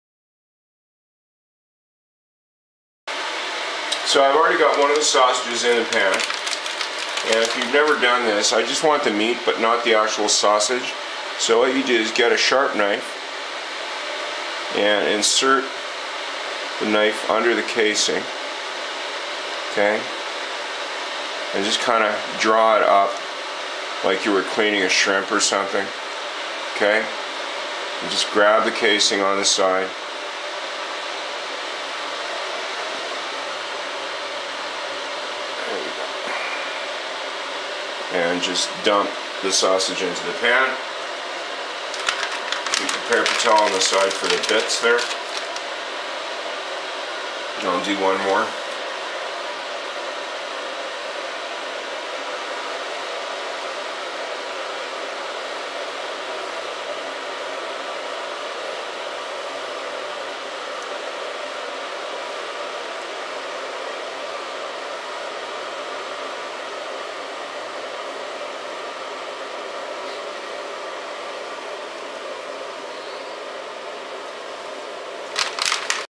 The Easy Way to Get the Meat Out of a Sausage (from 'Stromboli')
This technique is the fastest cleanest way to get the meat out when you need just the meat and not the sausage itself in a recipe.